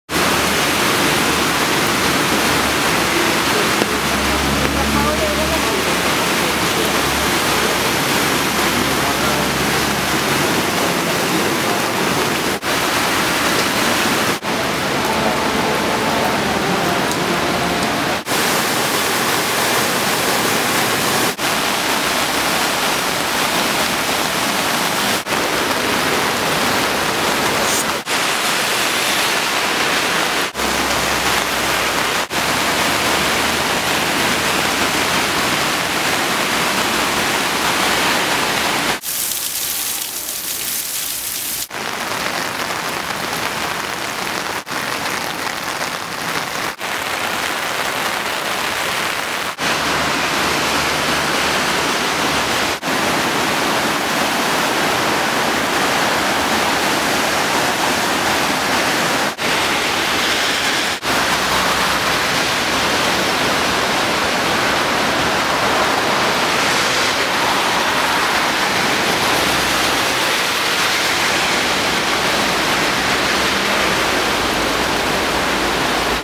Euritea Elgoibarren.m4v
Deba ibaia asko hasi da, Elgoibar parean, asteburu osoko euritearen ondorioz. 2011ko azaroaren 6a.